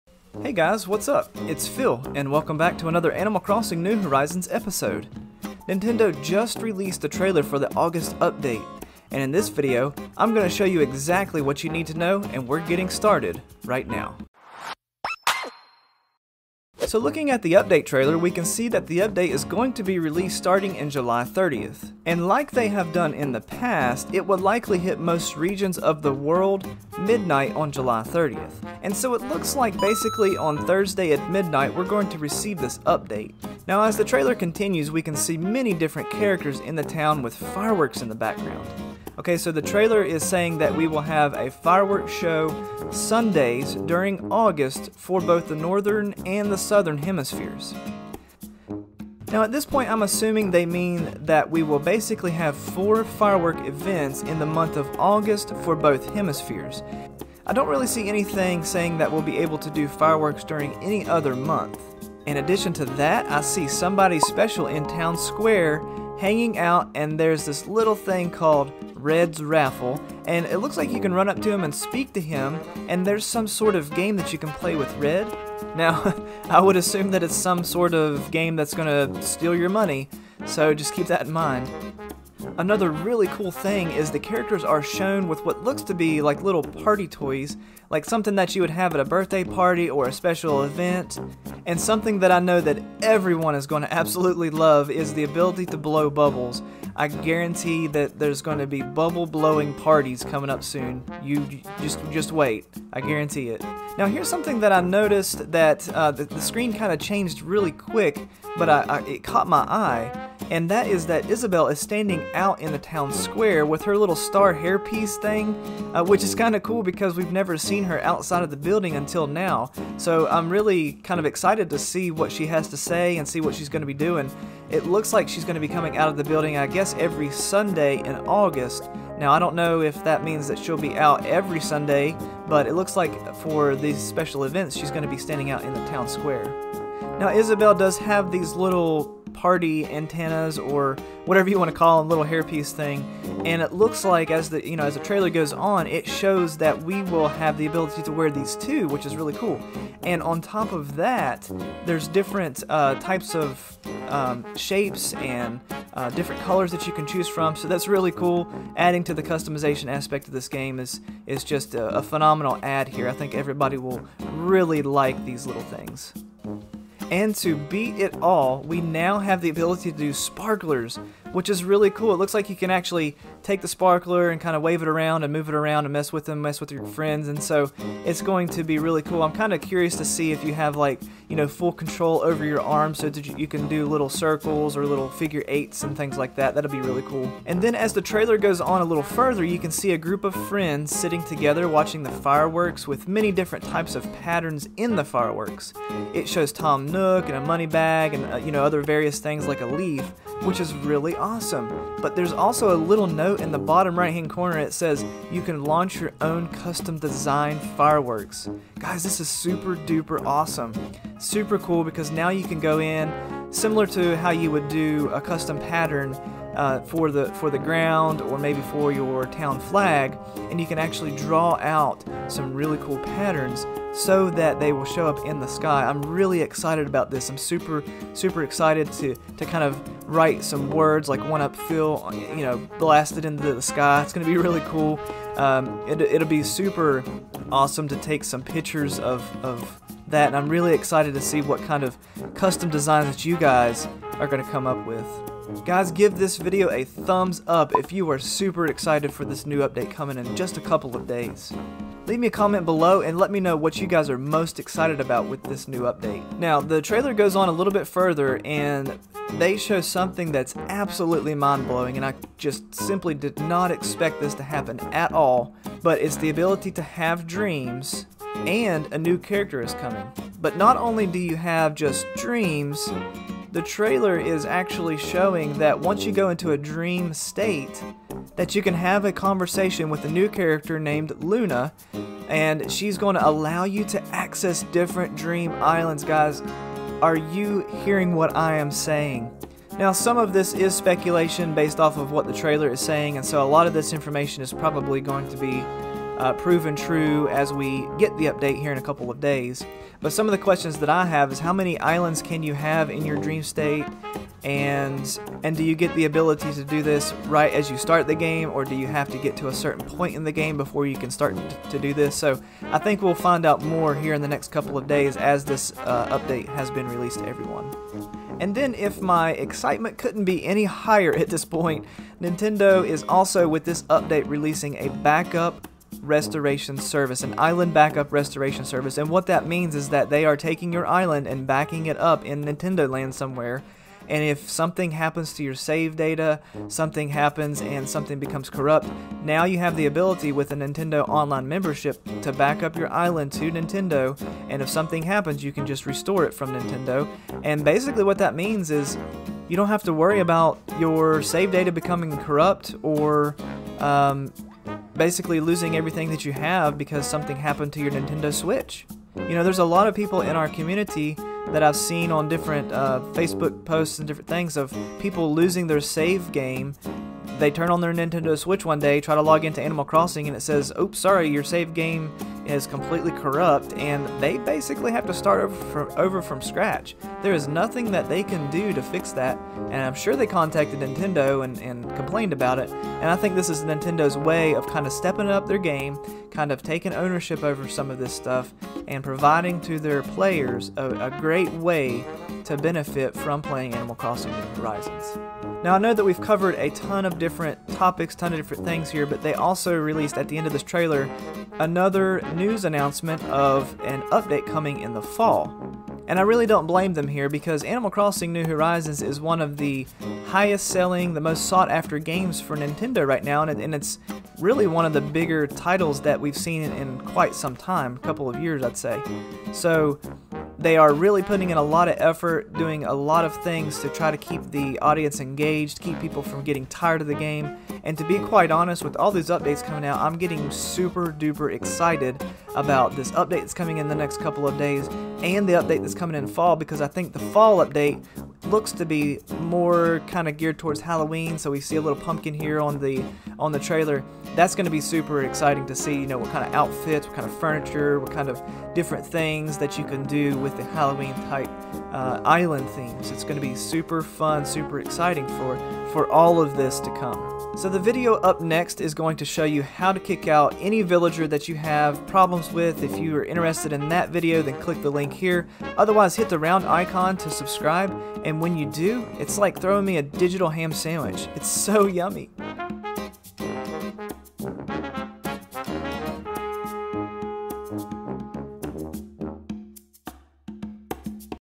MASSIVE UPDATE + New Features COMING to Animal Crossing New Horizons! (Summer Update Wave 2)
The new Switch Update for Animal Crossing New Horizons Summer Wave 2 Update reveals New Animal Crossing features and shops with Luna in Animal Crossing Fireworks, Dream Suites to visit other Animal Crossing Islands, and Island Backup and Restoration in Animal Crossing Gameplay coming to Animal Crossing 1.4 Update! As Summer Wave 2 Update in Animal Crossing is Coming Thursday, me as I talk through the new updates and features coming to Animal Crossing New Horizons!

►Nintendo Switch
►Nintendo Switch Pro Controller
►Capture Video for Nintendo Switch
►Microphone Stand